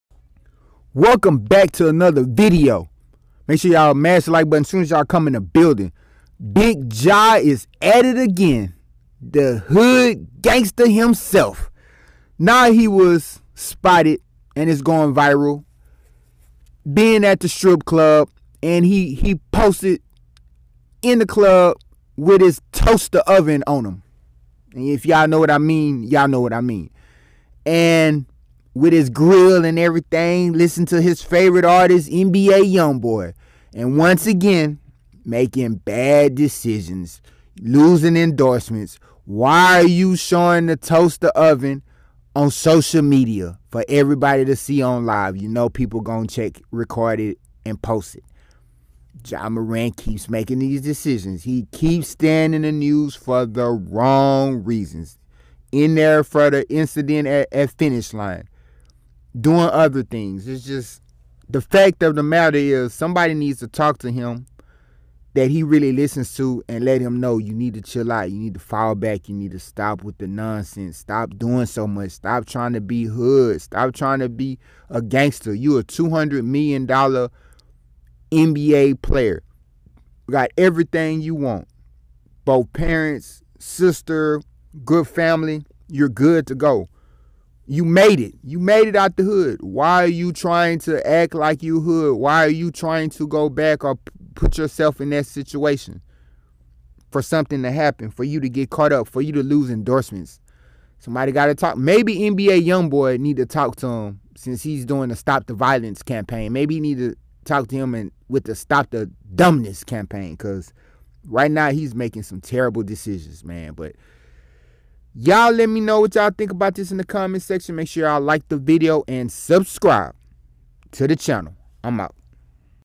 4KT Ja Morant Is Back At It Again In The Club ‼️ SMH
4KT Ja Morant Is Back At It Again In The Club ‼️ SMH

Somebody needs to talk to ja morant about staying out of the media for nonsense

This time ja was caught in the club holding something he shouldn't have